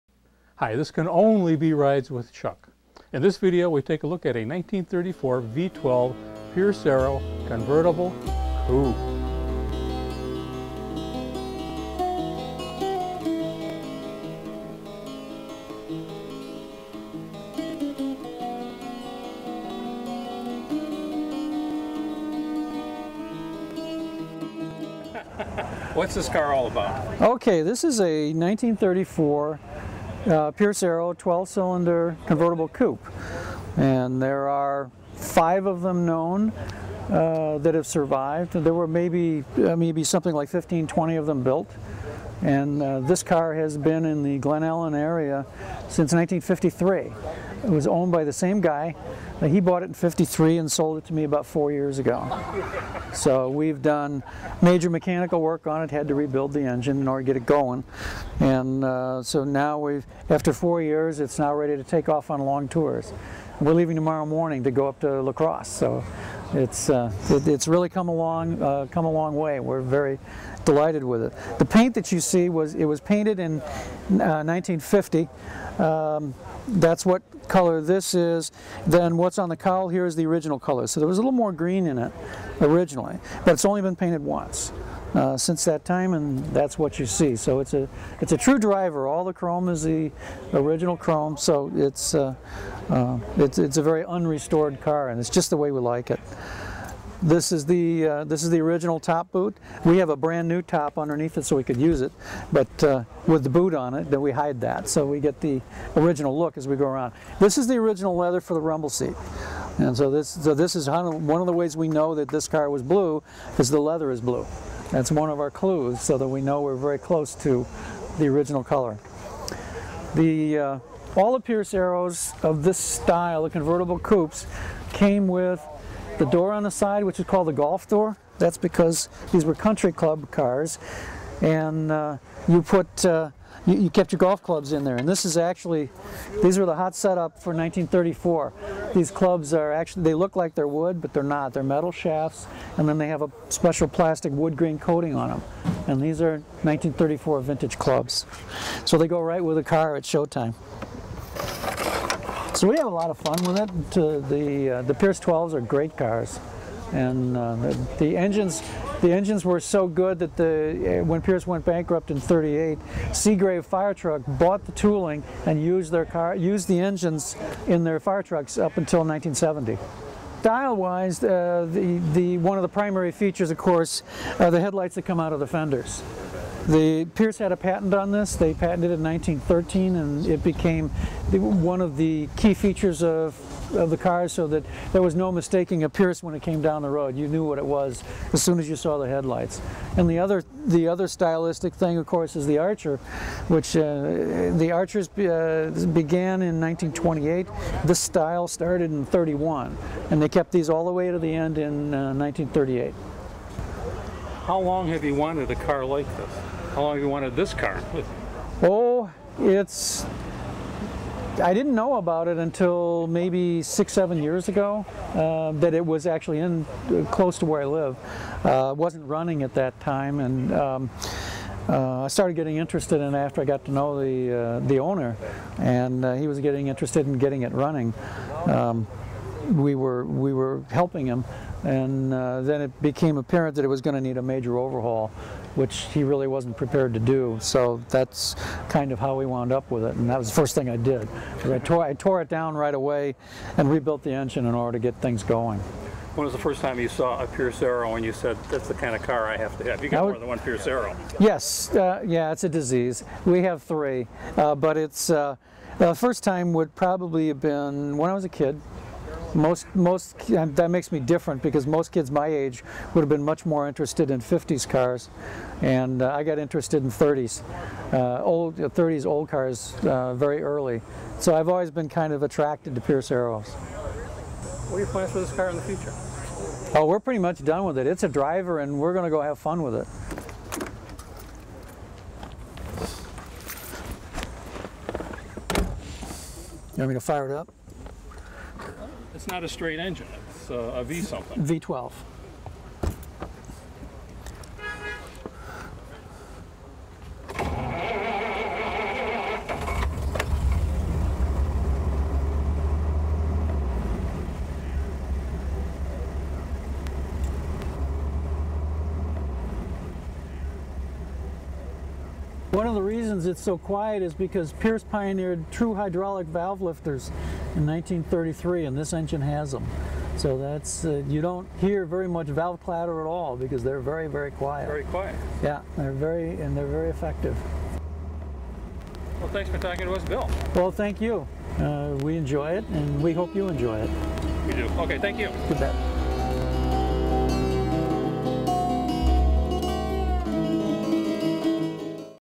1934 Pierce-Arrow Convertible Coupe
We talk with the owner of an original unrestored Pierce Arrow